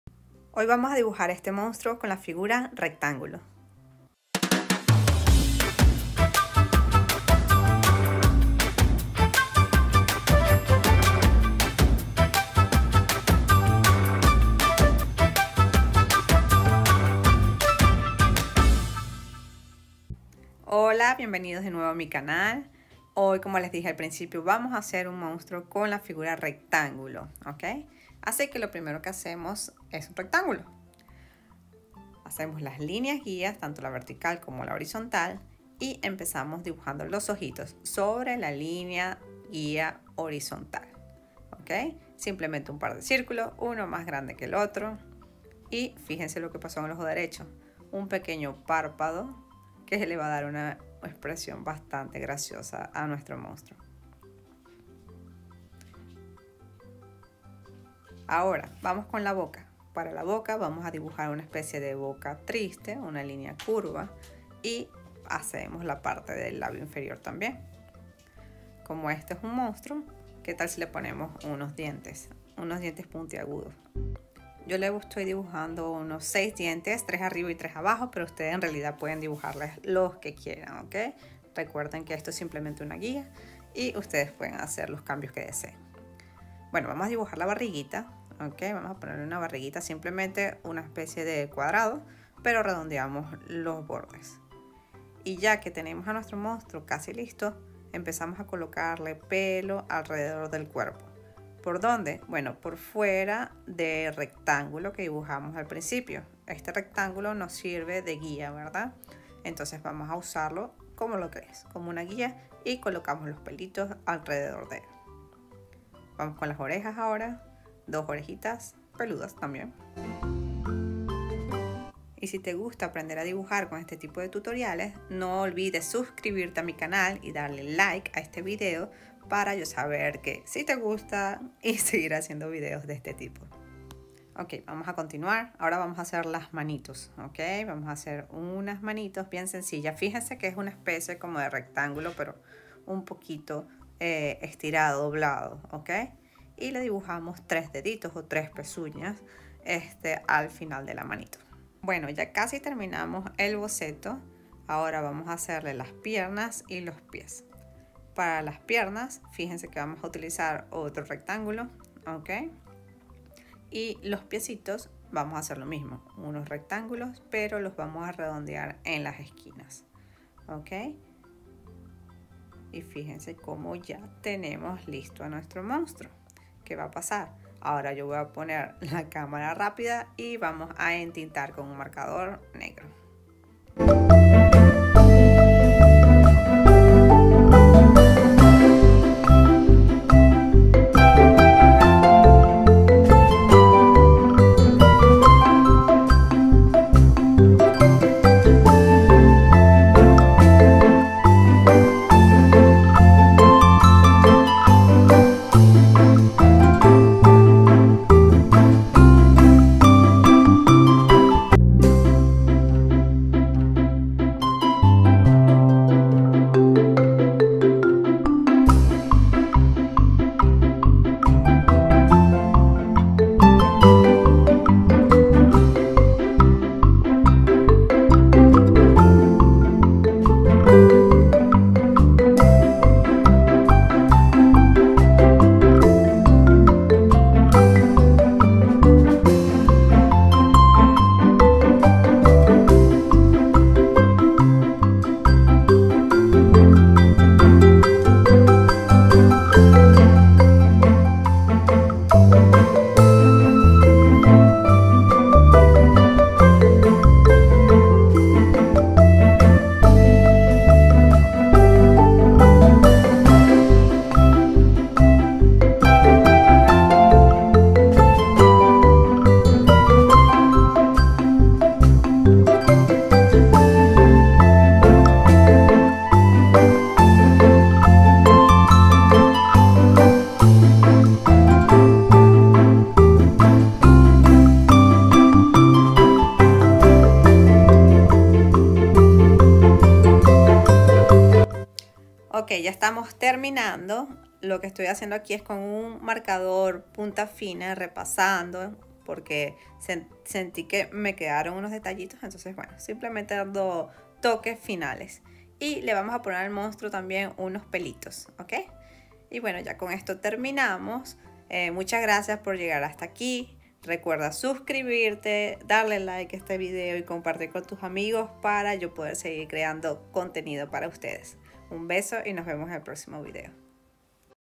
Como dibujar un monstruo fácil con la figura rectángulo | Dibujo de monstruo
Este es el segundo vídeo de la serie Dibujando con Figuras Geométricas, donde aprenderás a dibujar usando figuras geométricas básicas.

En este tutorial de dibujo utilizaremos la figura rectángulo para dibujar nuestro monstruo paso a paso. Luego te enseñaré cómo hacer el entintado y colorear nuestro monstruo usando marcadores Sharpie.

Un besote,
Dahyi